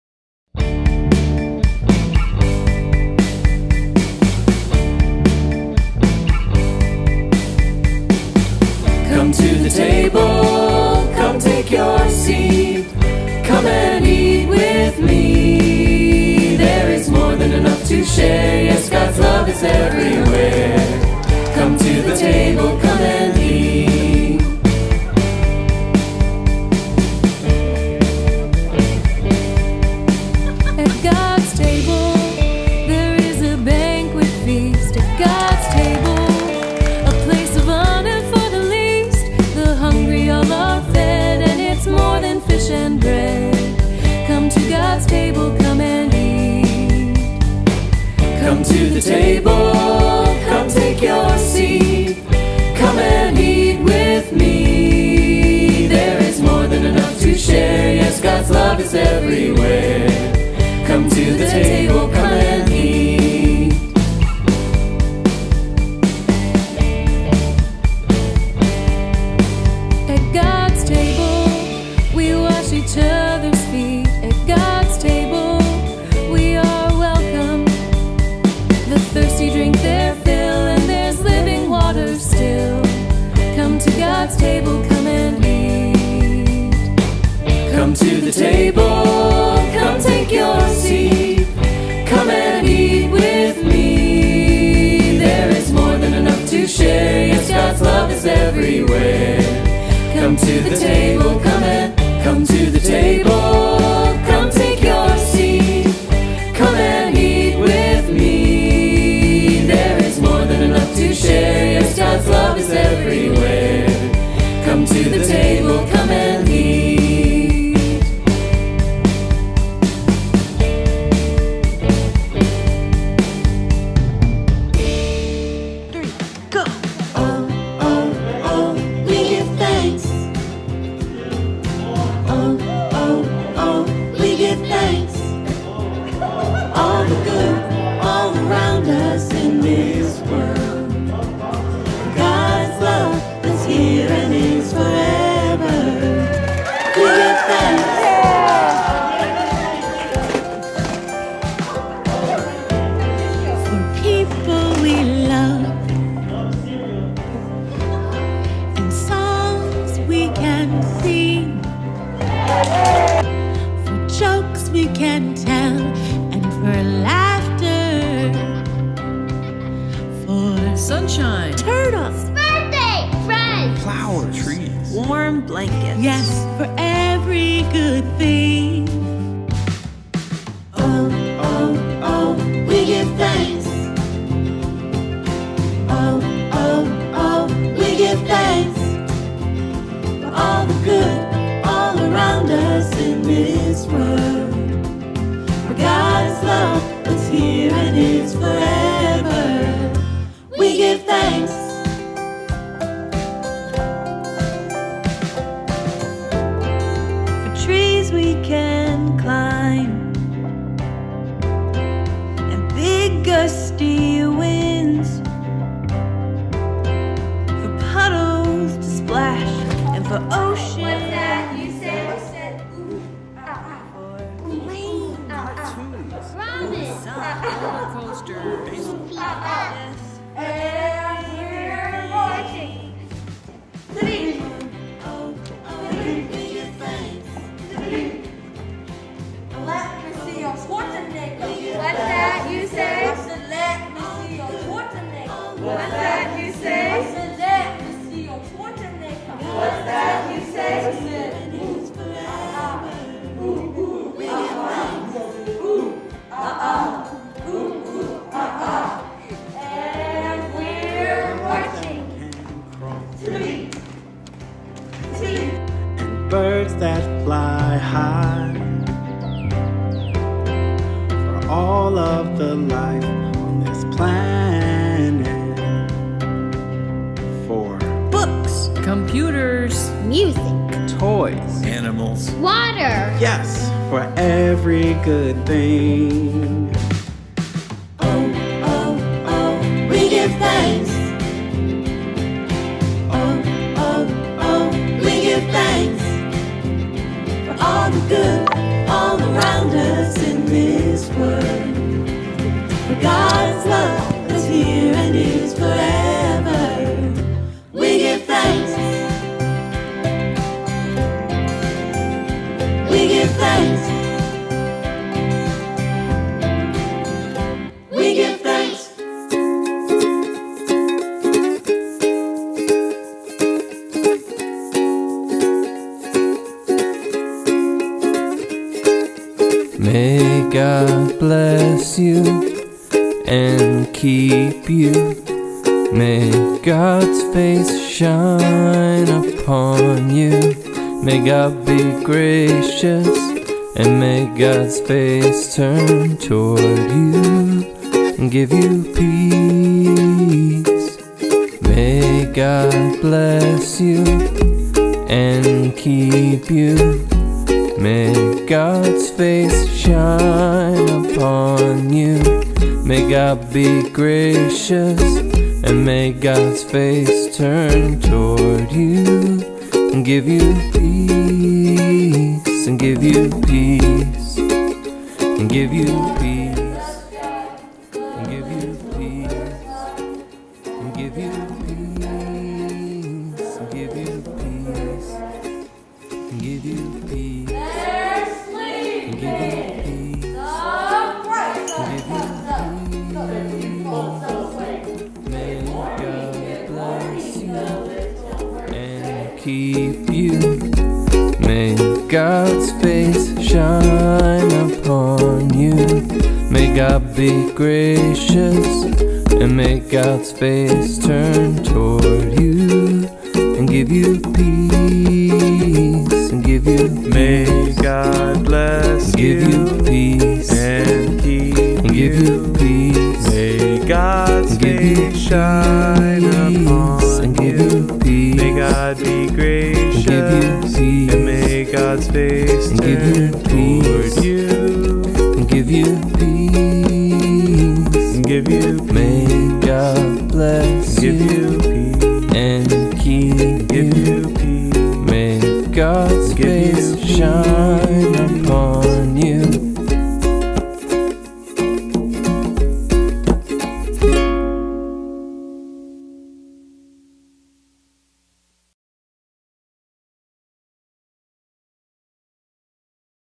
Vacation Bible School 2021 Highlights Reel
"COME TO THE TABLE"
Vacation Bible School Wrap Up Video
Co - Hosted by McPherson Church of the Brethren & First Mennonite Church